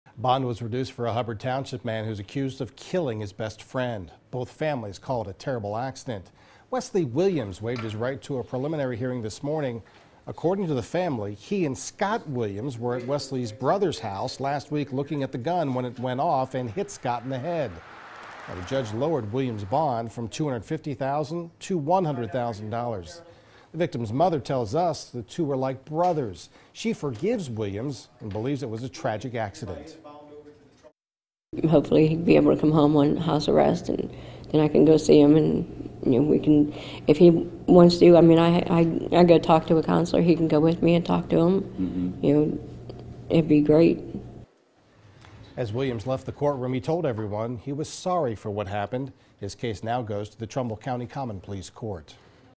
Bond lowered for Hubbard Twp. shooting death suspect
Wesley Williams's bond was lowered from $250,000 to $100,000 bond.